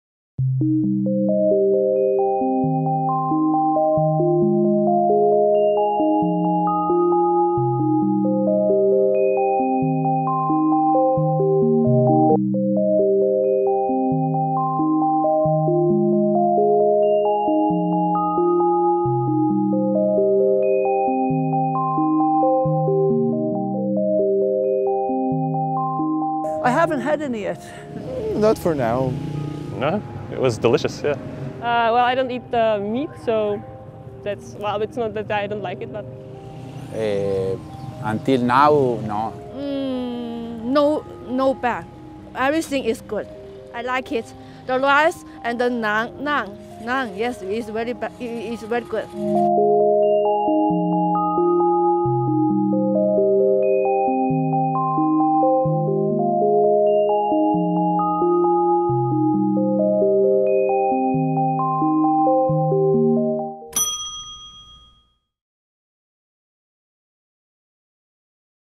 Worst Iranian food - Travel to Iran
In this video, we asked travelers about their worst food experience in Iran. What is your worst food experience? Please write here & share with all the people all over the world. Travel to Iran to re-discover unknown beauties.